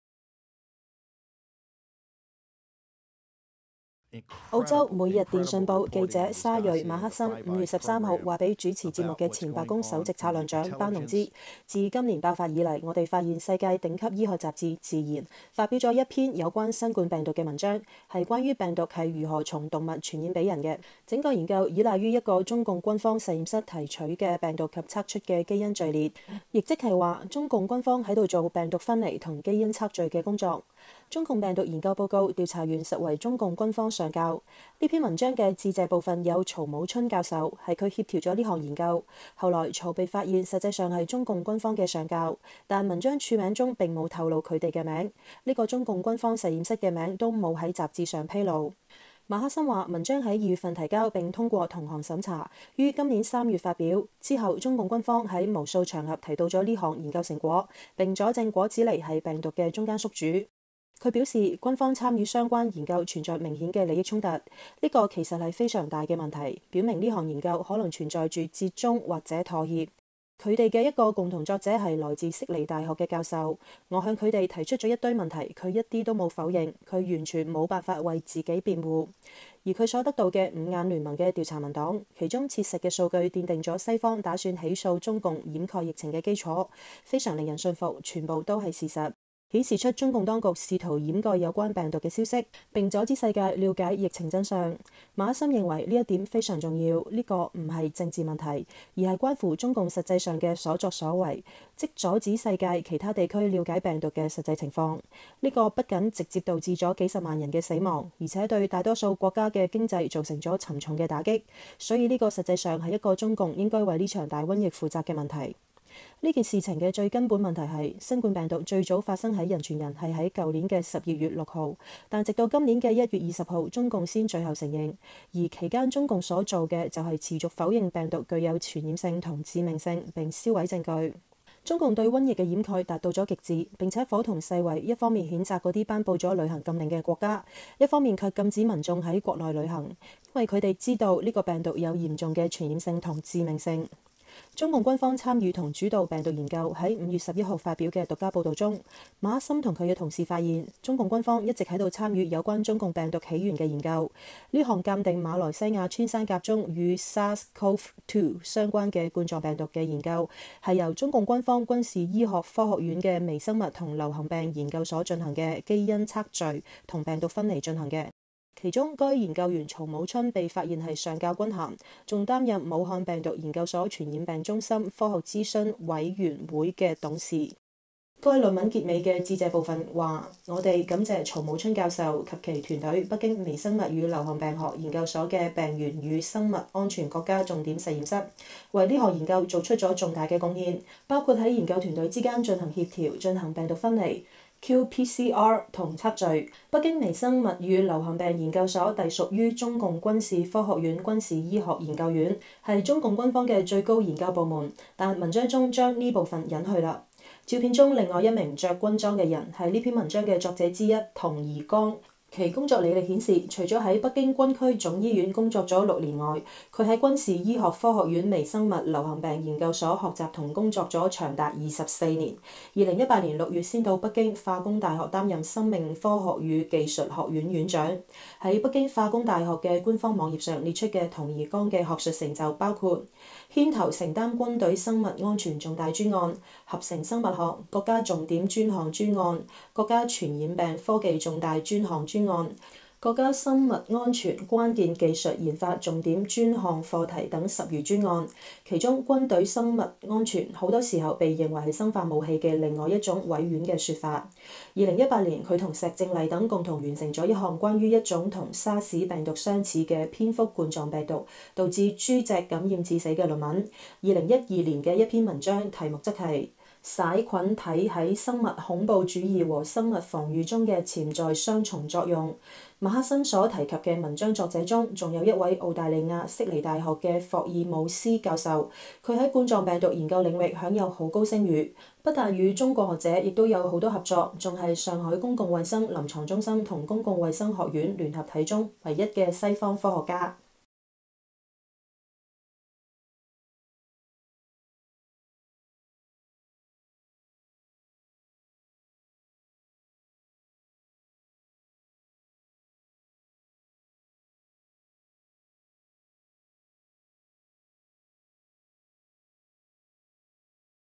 【焦點速遞】（字幕）澳洲《每日電訊報》記者莎瑞·馬克森5月13日訪談表示，中共在瘟疫初期隱瞞疫情傳染性和致命性長達六周，且中共軍方實驗室一直都在暗中參與甚至...
澳洲《每日電訊報》記者莎瑞·馬克森5月13日告訴主持節目的前白宮首席策略長班農（Steve Bannon），「自今年（中共病毒）爆發以來，我們發現世界頂級醫學雜誌《自然》發表了一篇有關新冠病毒（中共病毒）的文章，是關於病毒是如何從動物傳染給人，整個研究依賴於一個中共軍方實驗室提取的病毒及測出的基因序列」，也就是說，中共軍方在做病毒分離和基因測序的工作。

中共病毒研究報告 調查員實為中共軍方上校
「這篇文章的致謝部份有曹武春教授（排在首位），是他協調了這項研究，後來曹被發現實際上是中共軍方的上校。但文章署名中並沒有透露他們（被致謝者）的名字，這個中共軍方實驗室的名字也沒有在雜誌上披露。」馬克森說，文章在2月份提交並通過同行審查，於今年3月發表，之後中共官方在無數場合提到了這項研究成果，並佐證果子狸是病毒的中間宿主。

她表示，軍方參與相關研究存在明顯的利益衝突，「這其實是個非常大的問題，表明這項研究可能存在著折中或妥協。他們的（唯一）一個共同作者是來自悉尼大學的教授，我向他們（該大學）提出了一堆問題，他一點都沒有否認，他們完全無法為自己辯護。」
而她所得到的「五眼聯盟」的調查文檔，「其中切實的數據奠定了西方打算起訴中共掩蓋疫情的基礎，非常令人信服，全都是事實，顯示出中共當局試圖掩蓋有關病毒的消息，並阻止世界了解疫情真相」。
馬克森認為這一點非常重要，「這不是個政治問題，而是關乎中共實際上的所作所為，即阻止世界其它地區了解病毒的實際情況。這不僅直接導致了幾十萬人死亡，而且對大多數國家經濟造成了沉重打擊，所以這實際上是一個中共應該為這場大瘟疫負責的問題。」

「這件事情的最根本問題是，新冠病毒最早發生人傳人是在去年12月6日，但直到今年1月20日，中共才最後承認。而其間中共所做的，就是持續否認病毒具有傳染性和致命性，並銷毀證據。」

「中共對瘟疫的掩蓋達到了極致，並且夥同世衛一方面譴責那些頒布了旅行禁令的國家，一方面卻禁止民眾在國內的旅行，因為它們知道這個病毒有嚴重的傳染性和致命性。」

中共軍方參與和主導病毒研究
在5月11日發表的獨家報道中，馬克森和她的同事發現，中共軍方一直在參與有關中共病毒起源的研究。這項「鑑定馬來亞穿山甲中與SARS-CoV-2相關的冠狀病毒」的研究，是由中共軍方軍事醫學科學院的微生物和流行病研究所進行的「基因測序」和「病毒分離」進行的。其中，該研究員曹武春被發現是上校軍銜，還擔任武漢病毒研究所傳染病中心科學諮詢委員會的董事。

該論文結尾的致謝部份說，「我們感謝 教授……（等其他4位博士）及其團隊北京微生物與流行病學研究所的病原與生物安全國家重點實驗室，為這項研究做出了重大貢獻，包括在研究團體之間進行協調、進行病毒分離、qPCR（定量即時聚合酶鏈式反應）和測序。」北京微生物與流行病研究所隸屬於中共軍事科學院軍事醫學研究院，是中共軍方的最高研究部門，但文章中把這部分隱去了。

照片中另外一名穿軍裝的人，是這篇文章的作者之一童貽剛。其工作履歷顯示，除了在北京軍區總醫院工作了6年外，他在軍事醫學科學院微生物流行病研究所學習和工作了長達24年，2018年6月才到北京化工大學擔任生命科學與技術學院院長。

在北京化工大學的官方網頁上列出的童貽剛的學術成就包括：牽頭承擔軍隊生物安全重大專案、「合成生物學」國家重點專項專案、國家傳染病科技重大專項專案、國家「生物安全關鍵技術研發」重點專項課題等十餘專案。
其中「軍隊生物安全」，很多時候被認為是生化武器的另一種委婉說法。
2018年，他與石正麗等共同完成了一項關於一種與沙士（SARS）病毒相似的蝙蝠冠狀病毒（SADS-CoV）導致豬隻感染致死的論文。2012年的一篇文章題目則是「噬菌體在生物恐怖主義和生物防禦中的潛在雙重用途」。
馬克森所提及的文章作者中，還有一位澳大利亞悉尼大學的霍爾姆斯（Edward Holmes）教授，他在冠狀病毒研究領域享有很高聲譽，不但與中國學者也有很多合作，還是上海公共衛生臨床中心和公共衛生學院聯合體中唯一的西方科學家。
責任編輯：seven.xue

📕新紀元五月刊現已上市！🎉